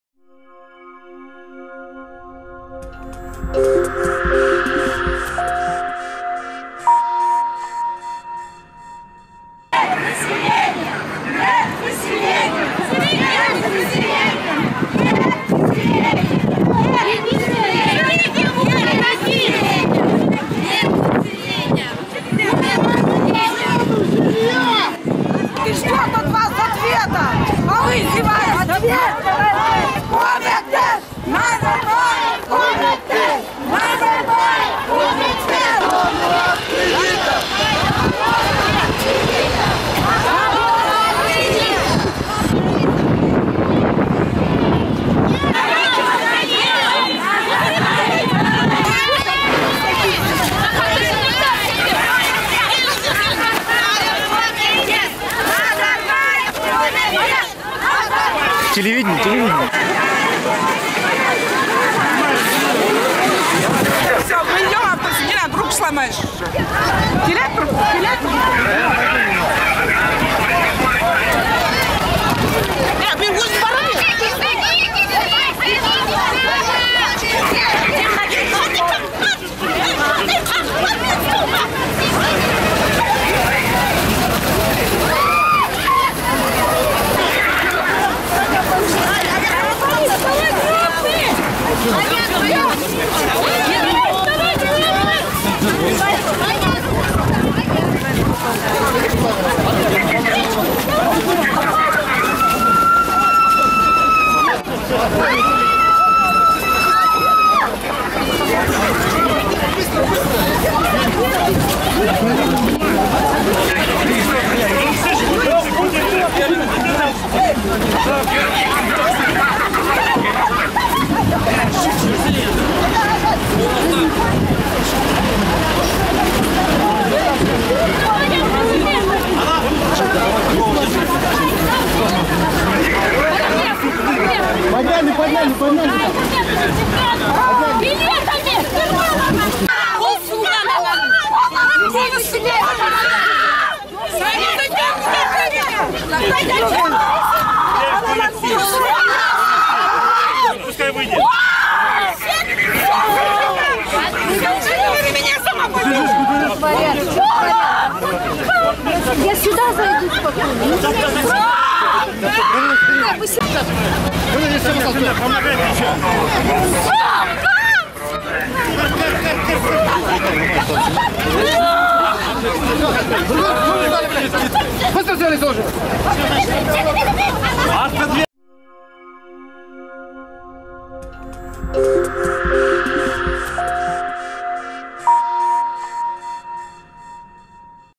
Митинг ипотечников у здания парламента и правительства Казахстана / 1612
Митинг проблемных заемщиков у здания правительства и парламента Казахстана закончился разгоном митингующих.